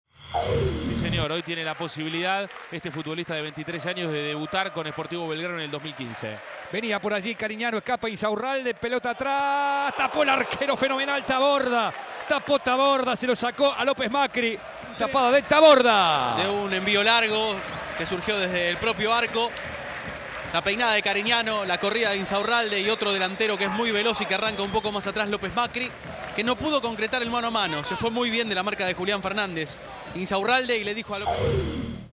Espectacular tapada de Taborda. Sp. Belgrano 0 - Chacarita 0. Fecha 1. B Nacional 2015. FPT.
Mirá la gran atajada del arquero de Chaca en la primera jugada del encuentro ante el delantero López Macri de Sportivo Belgrano.